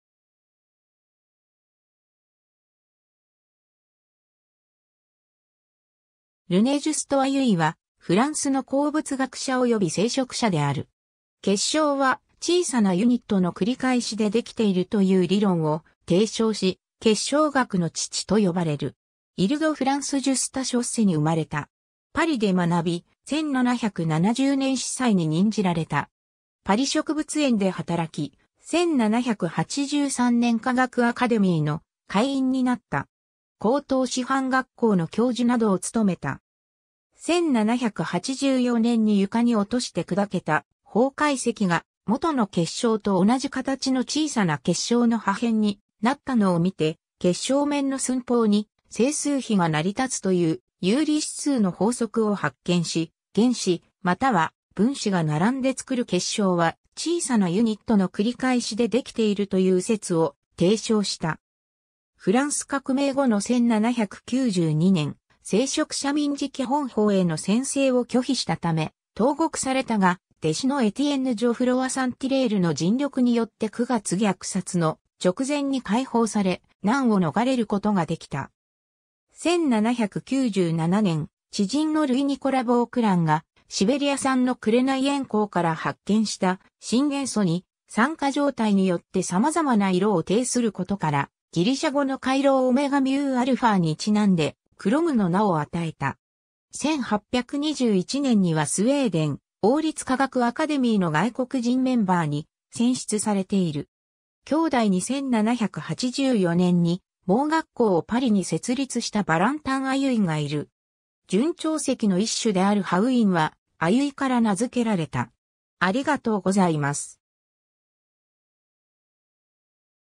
ルネ＝ジュスト・アユイ
高等師範学校_(フランス)の教員
国立自然史博物館_(フランス)の人物
ルネ＝ジュスト・アユイ（René Just Haüy、1743年2月28日 - 1822年6月3日）は、フランスの鉱物学者および聖職者である。
「結晶は小さなユニットの繰り返しでできている」という理論を提唱し、「結晶学の父」と呼ばれる。
 イル＝ド＝フランス（後のオワーズ県）サン＝ジュスタン＝ショッセに生まれた。
パリで学び、1770年司祭に任じられた。
パリ植物園で働き、1783年科学アカデミーの会員になった。
高等師範学校の教授などを務めた。
 1784年に床に落として砕けた方解石（カルサイト）が、元の結晶と同じ形の小さな結晶の破片になったのを見て、結晶面の寸法に整数比が成り立つという「有理指数の法則」を発見し、原子または分子が並んで作る結晶は小さなユニットの繰り返しでできているという説を提唱した。
 フランス革命後の1792年、聖職者民事基本法への宣誓を拒否したため投獄されたが、弟子のエティエンヌ・ジョフロワ・サンティレールの尽力によって九月虐殺の直前に解放され、難を逃れることができた。
 1797年、知人のルイ＝ニコラ・ヴォークランがシベリア産の紅鉛鉱から発見した新元素に、酸化状態によってさまざまな色を呈することからギリシャ語の χρωμα（chrōma、色）にちなんでクロムの名を与えた。
1821年にはスウェーデン王立科学アカデミーの外国人メンバーに選出されている。
 兄弟に1784年に盲学校をパリに設立したヴァランタン・アユイ（1745年 - 1822年）がいる。
 準長石の一種である haüyne（アウイン、和名：藍方石、(Na,Ca)4-8Al6Si6O24(SO4,S)1-2）は、アユイから名付けられた。